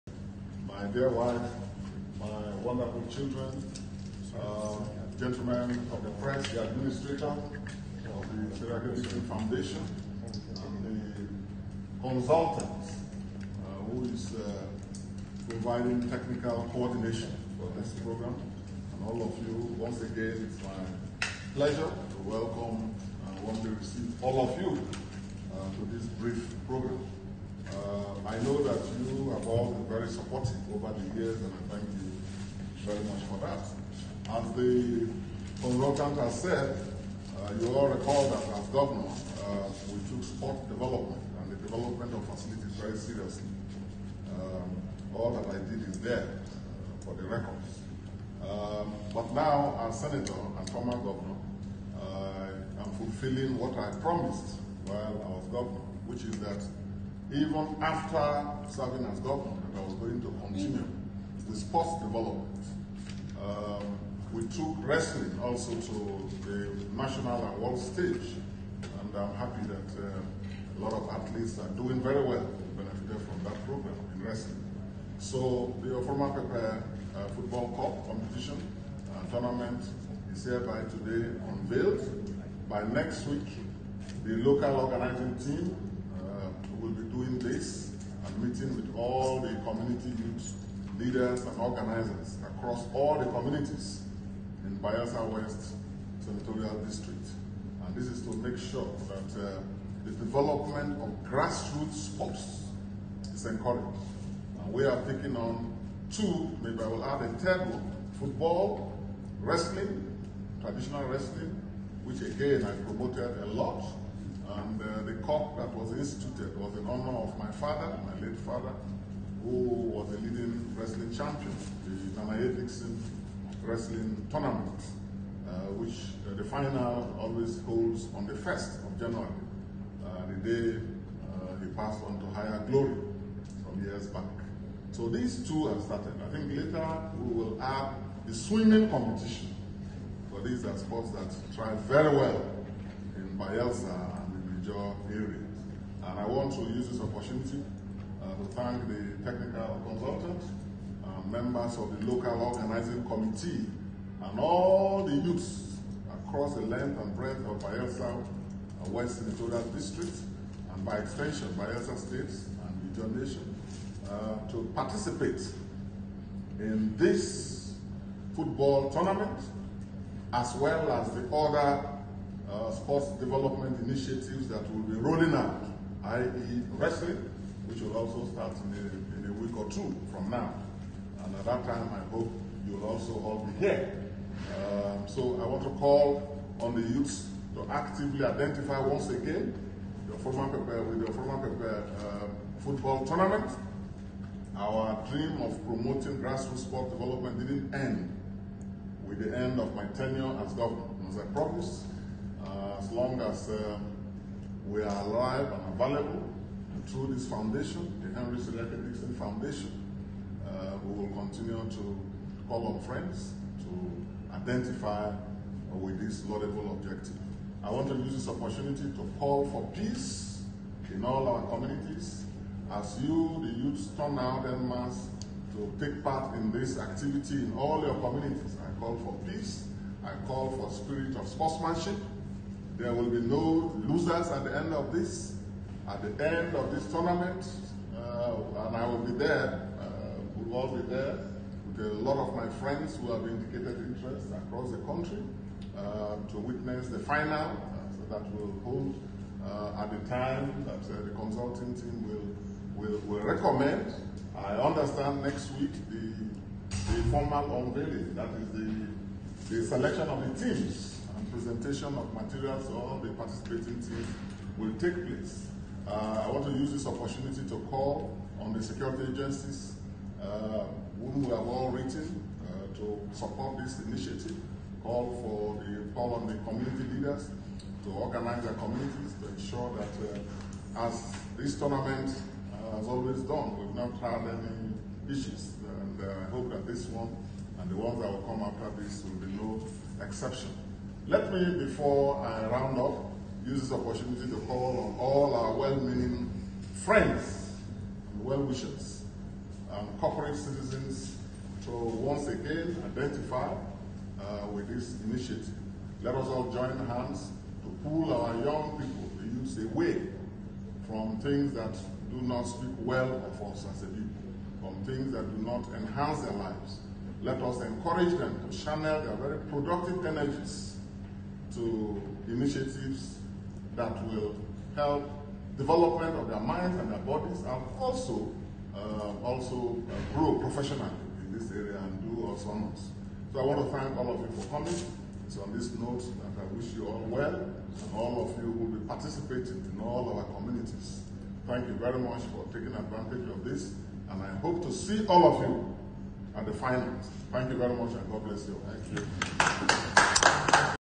Senator Henry Seriake Dickson Unveils Ofurumapepe Cup in Abuja on 12.11.2021
Senator Henry Seriake Dickson launches Ofrumapepe Cup in Abuja with a strong appeal for Bayelsa Youth in the Bayelsa West Senatorial District to participate fully. Also appealed to the security agencies to be  involved. Senator Dickson seen here flanked by his immediate family members.